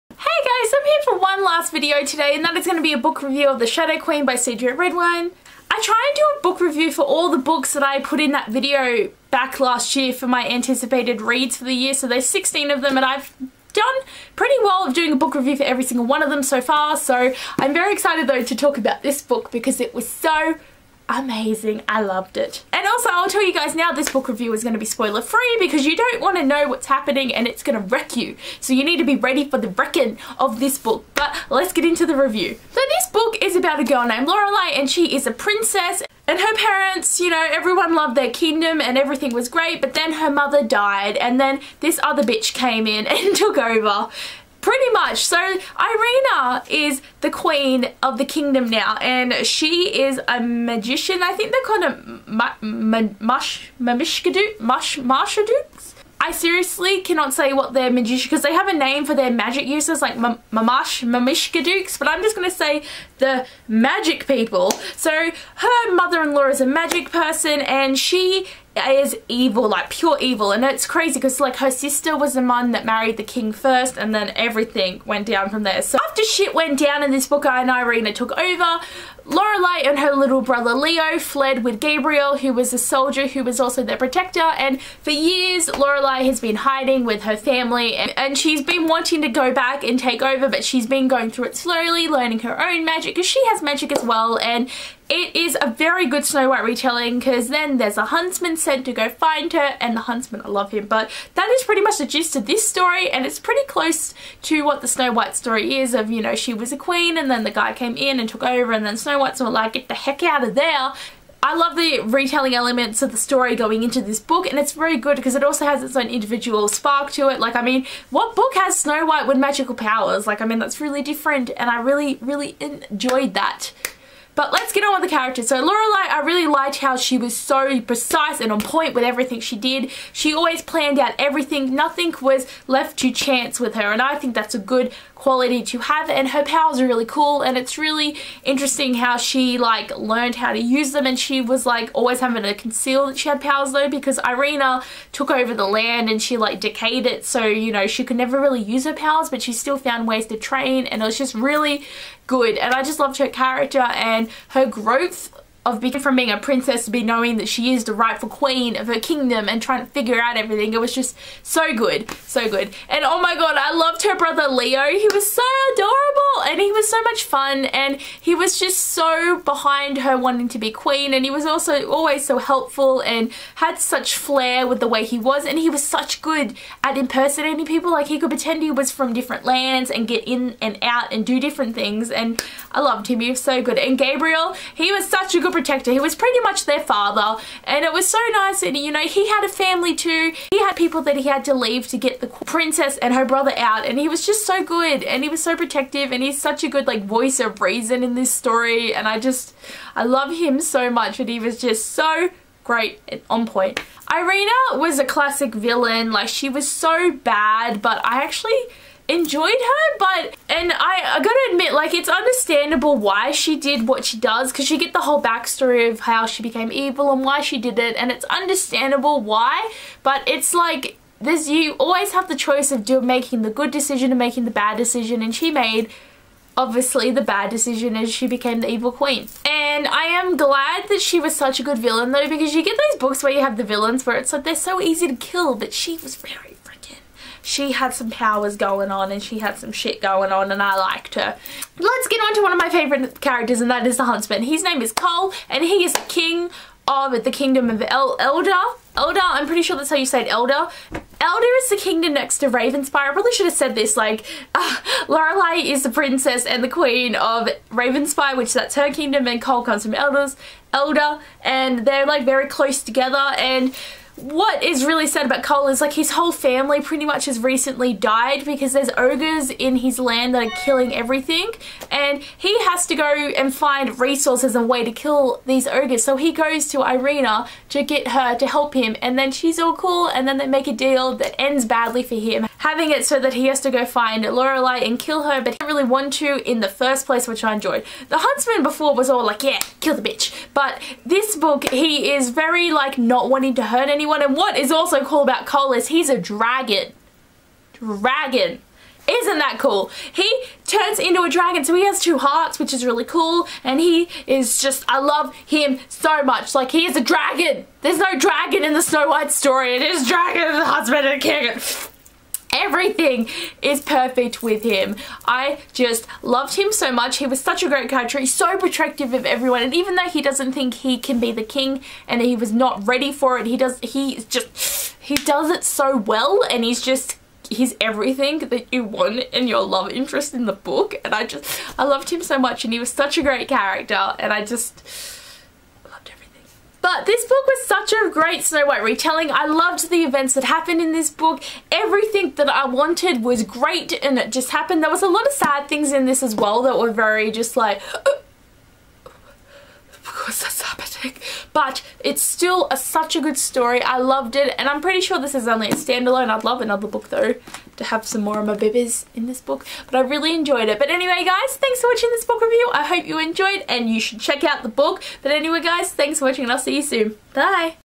The Shadow Queen Review
Hey guys

I really enjoyed this book. It was a really good retelling of Snow White and a great story.

Here are my links:

Instragram - abbeyleec_the_booklover